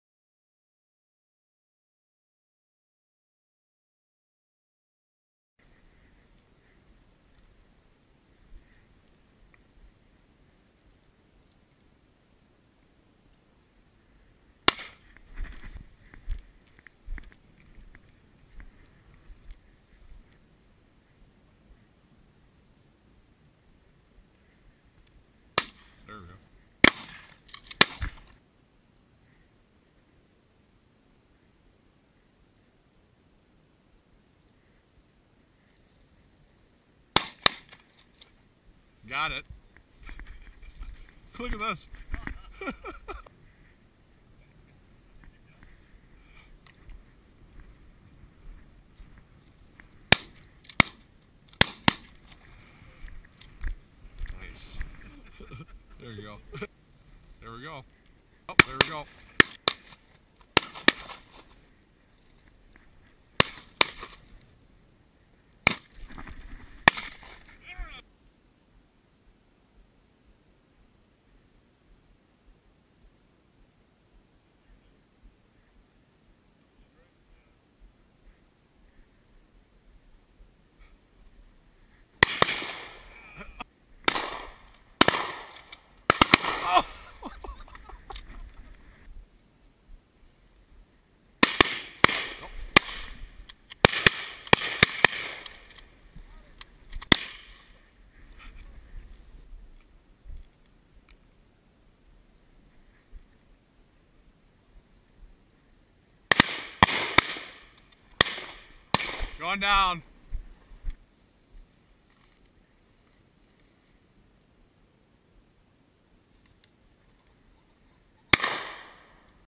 Illinois December Pheasant Hunt
Lots of shooting on this Illinois December pheasant hunt! Tower Shoot Jan. 31st!